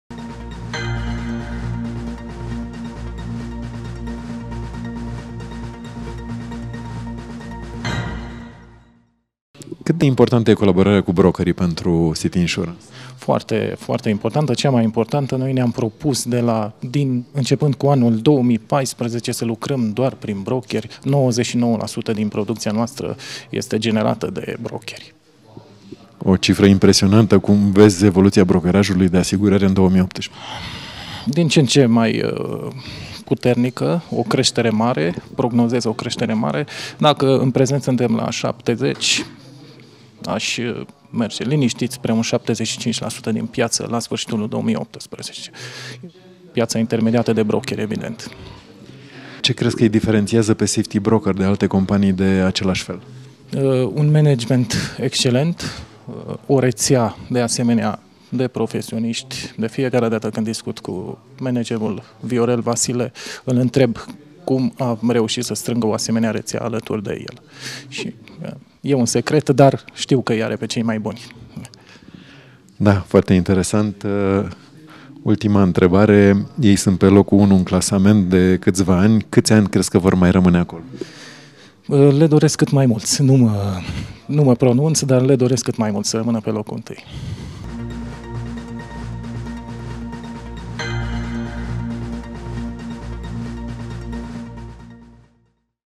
Marea Adunare a Partenerilor SAFETY Broker 2018 Mircea BUZERA Director Vanzari, CITY Insurance
23-25 martie, Cheile Gradistei Interviu realizat de Sergiu COSTACHE. Video original aici